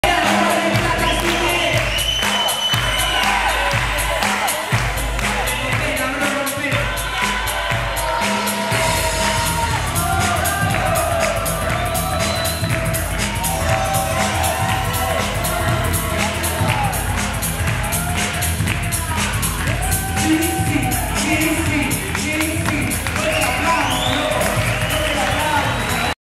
Bboys zarko | MDQ CYPHER 012 COMPETENCIA DE BREAKDANCE 2VS2
bboys zarko

DOMINGO 20 DE MAYO 2012 @ Mar del Plata
competencia breakdance